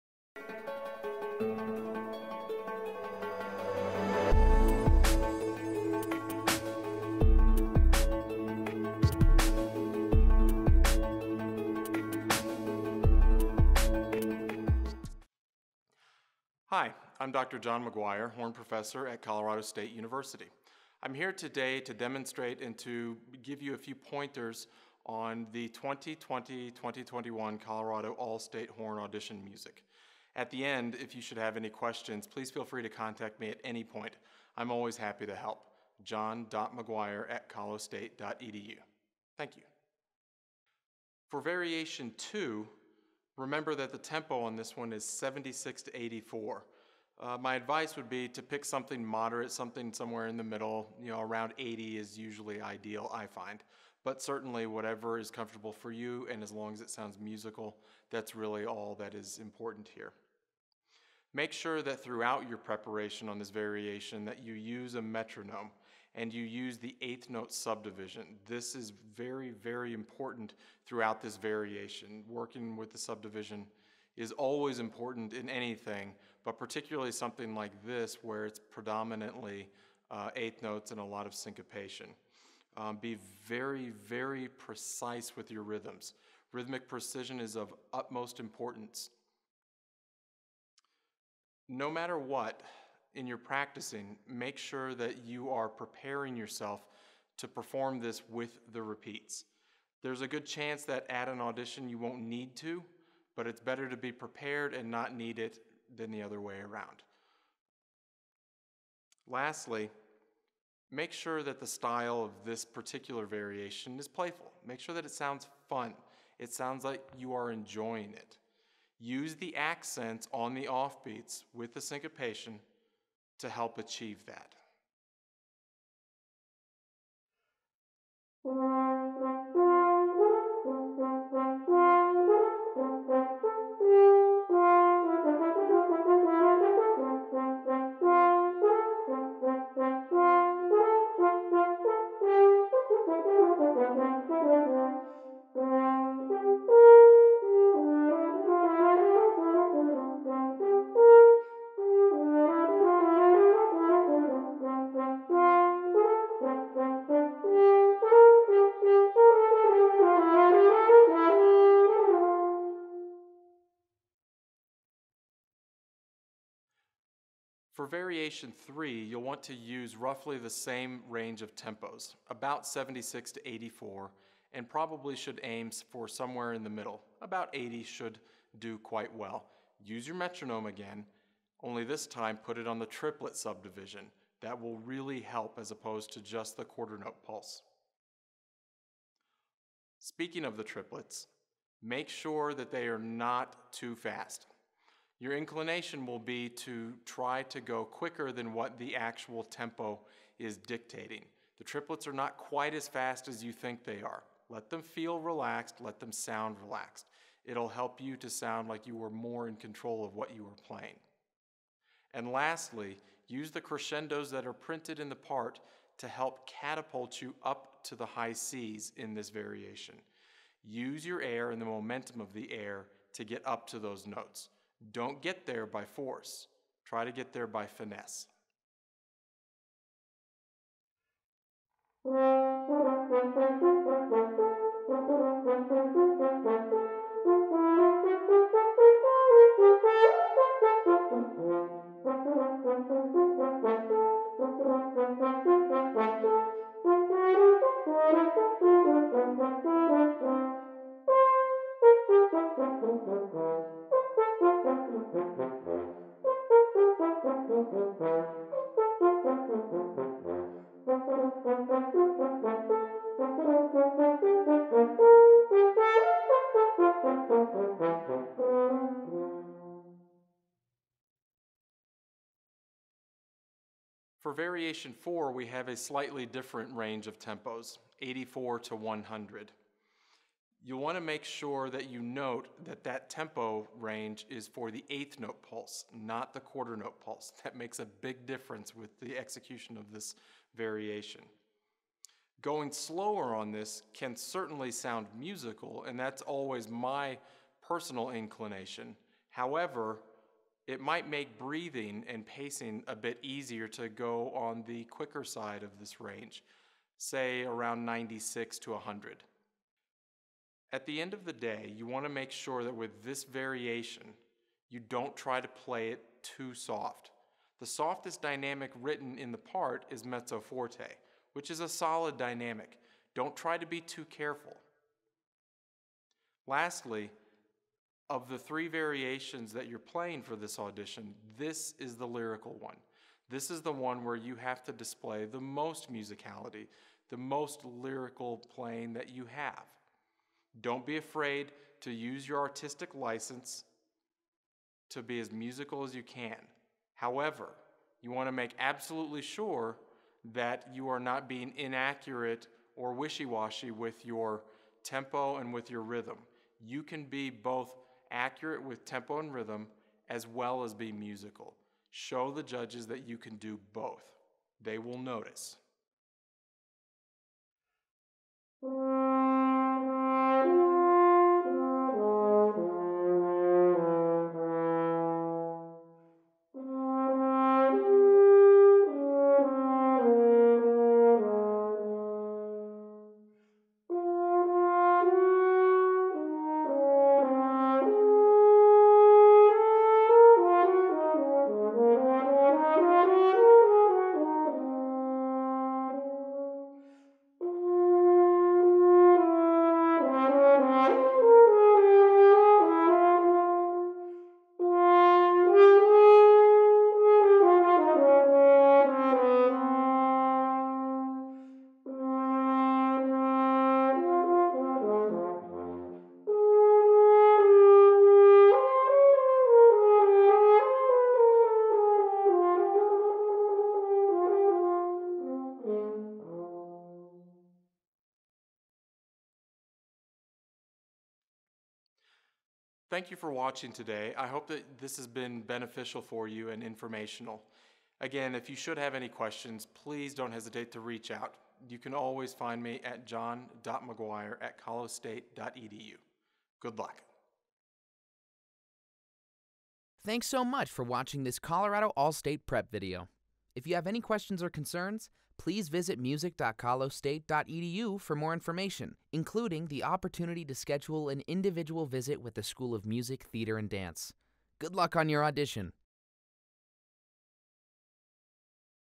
2020-2021 Colorado All-State Auditions: Horn, John McGuire
In this session, Colorado State University French Horn professor, Dr. John McGuire, demonstrates both practice and performance techniques helpful for students preparing for the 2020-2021 Colorado All-State horn audition.